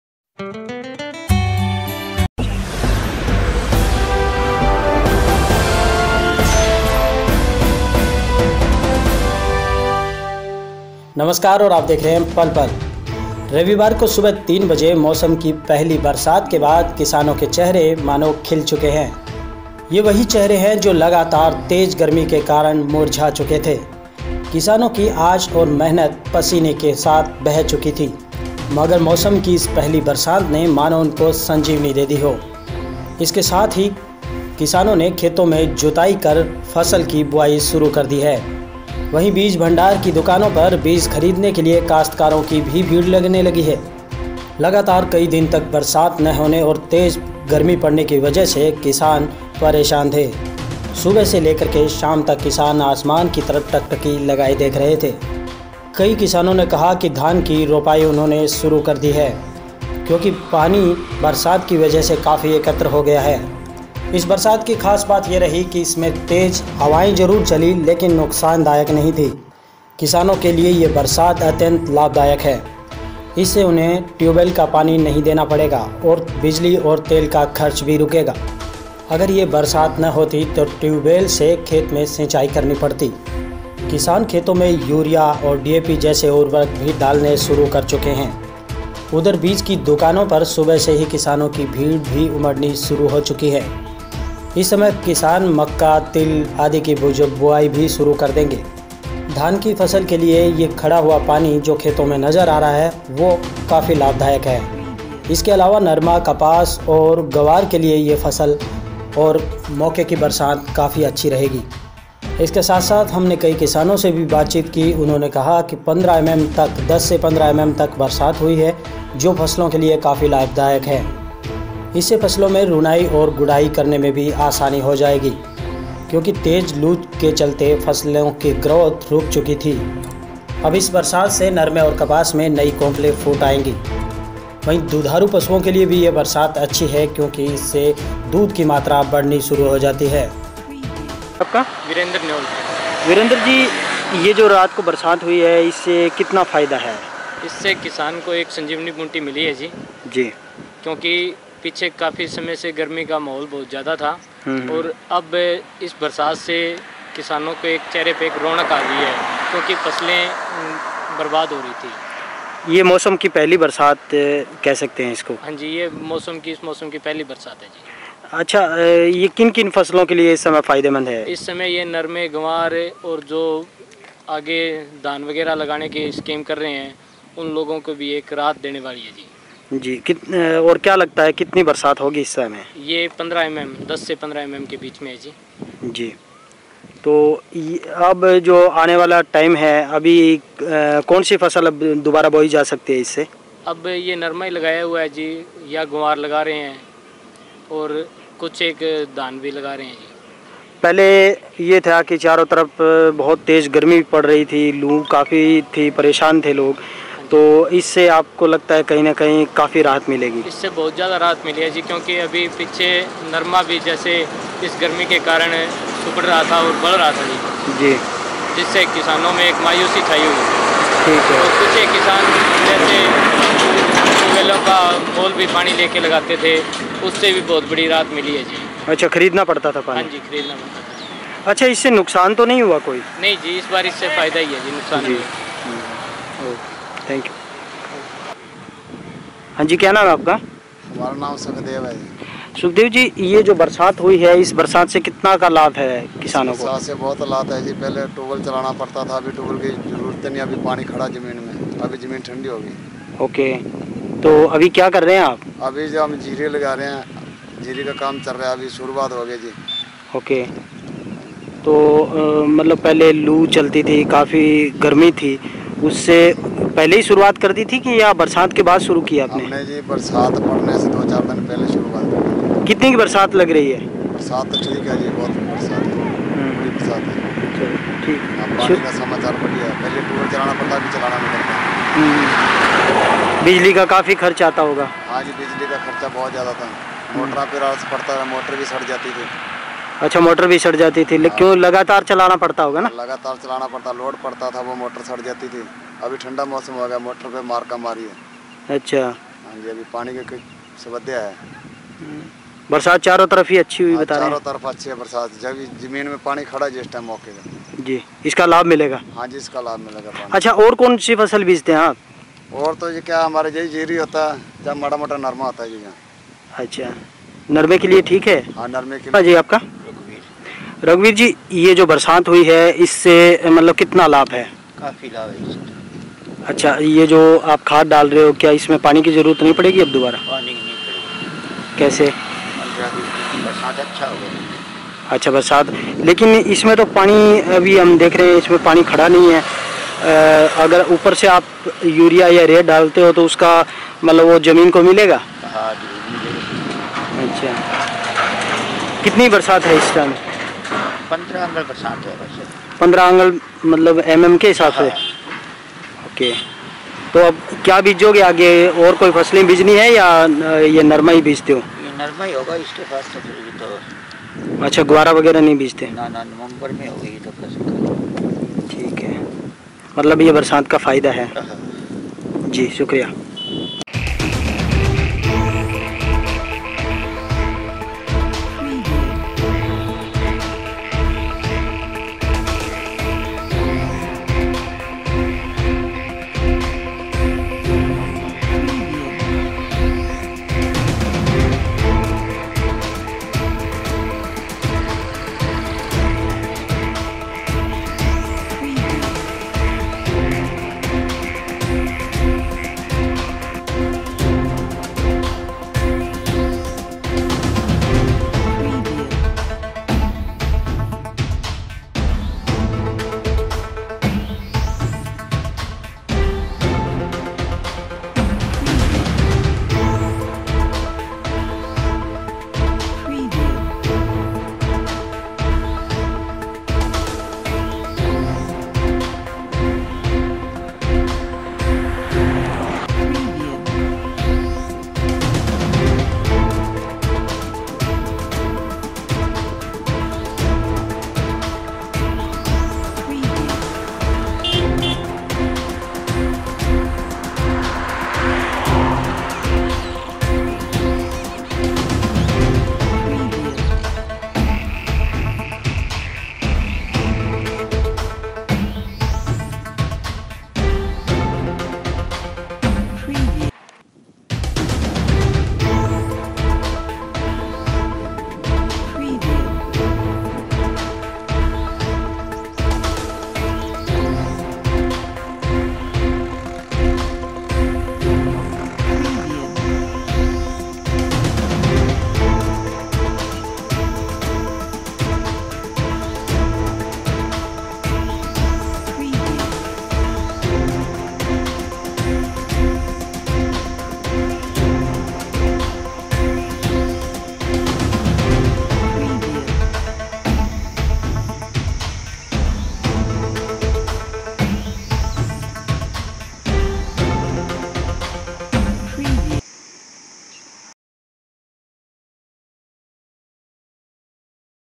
जमीं,सुलगता आसमान-मौसम की पहली बरसात-बरसी राहत, क्या कहते किसानों ?
barish तपती जमीं,सुलगता आसमान-मौसम की पहली बरसात, क्या कहते किसानों ?